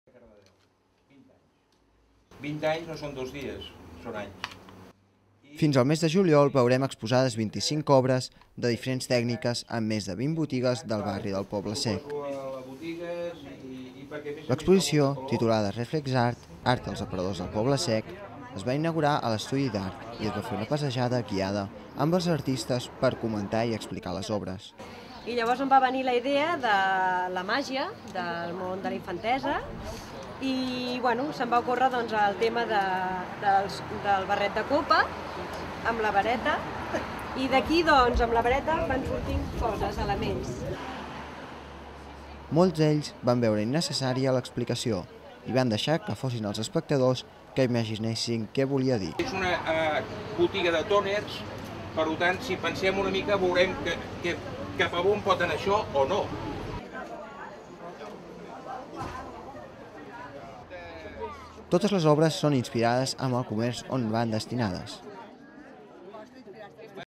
Jordi Aligué als aparadors del Poble Sec
Ja fa 20 anys que l'Estudi d'art Jordi Aligué - Anna Bellvehí va obrir les seves portes. També fa dos anys que est troba situat al poble sec. Els artistes han volgut commemorar la data i ho han fet amb la col·laboració de les botigues del barri.